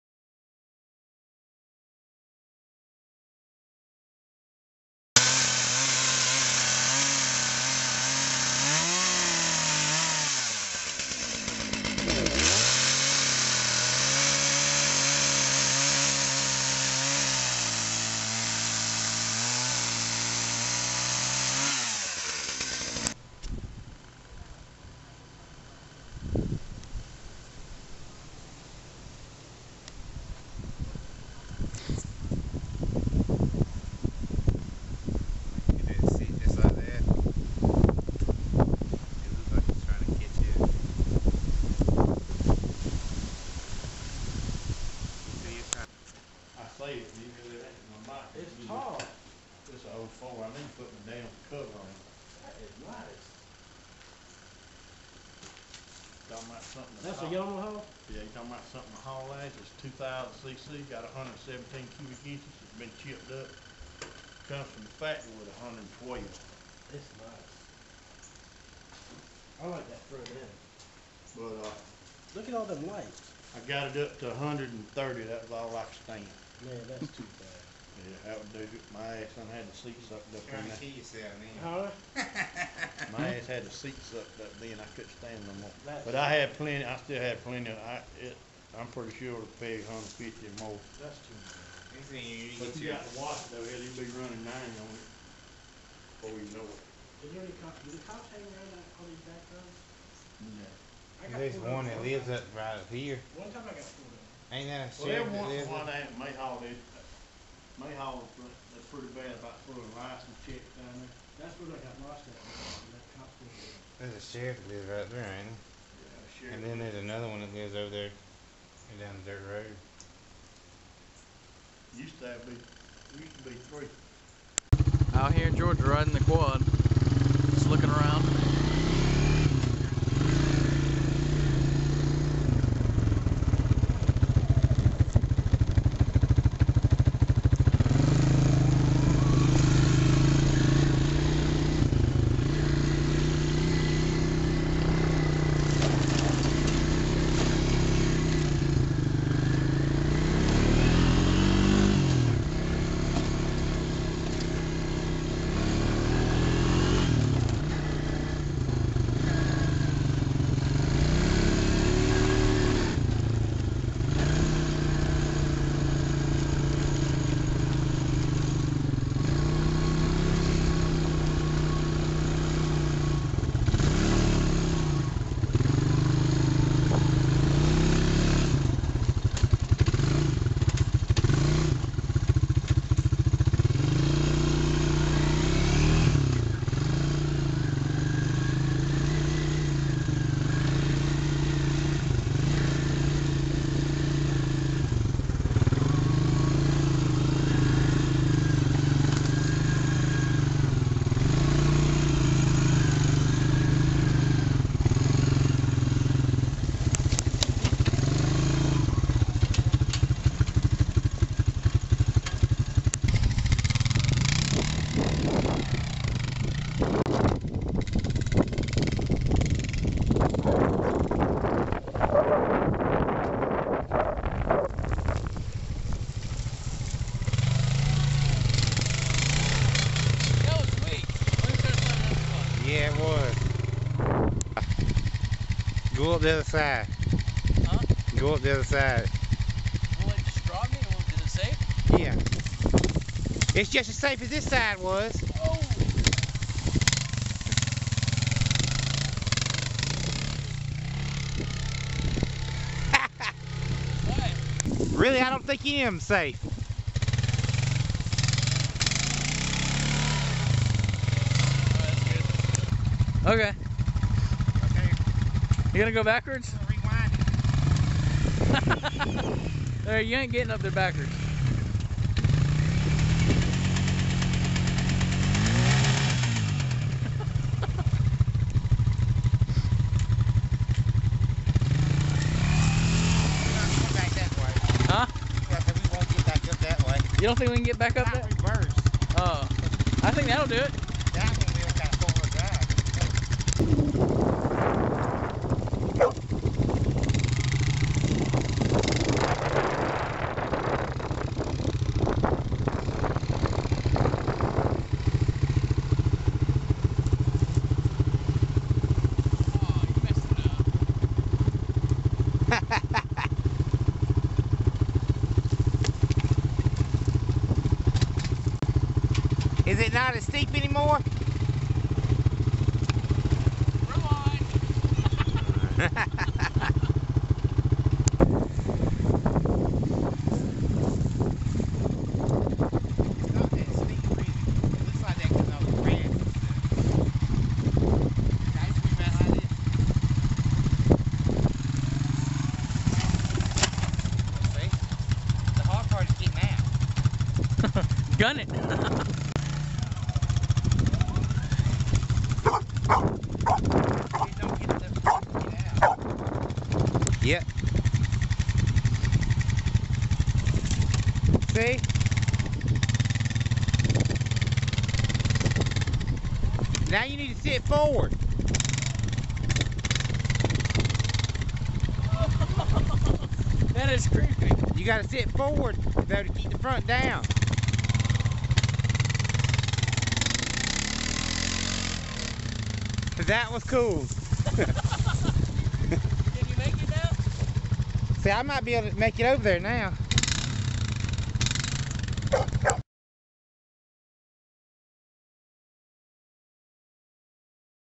trip to georgia
This is my trip to georgia,riding quads,makin fires,and more